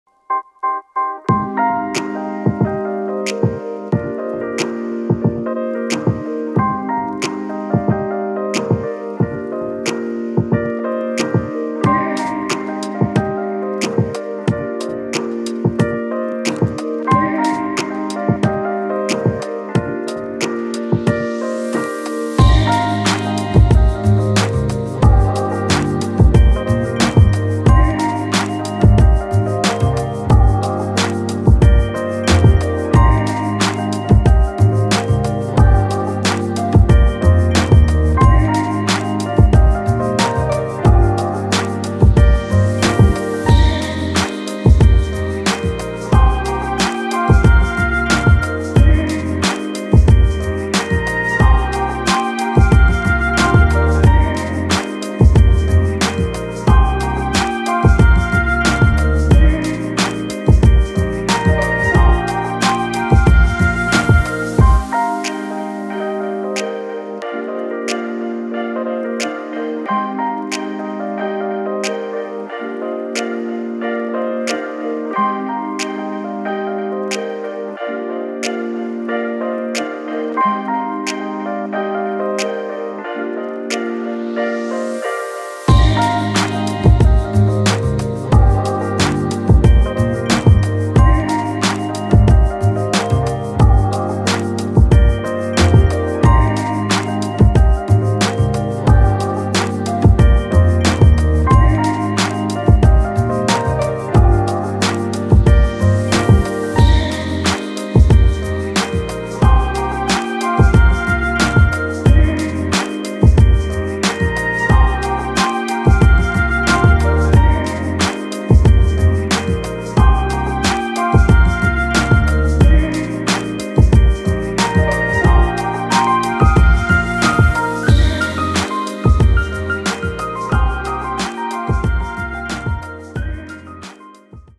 Day to Night - Breathtaking Aerial Drone Video - Part 1 - Watch in HD
I flew my drone the exact same route hours apart at various spots in Los Angeles, California. This is Part one of a three part series. Where should I fly next?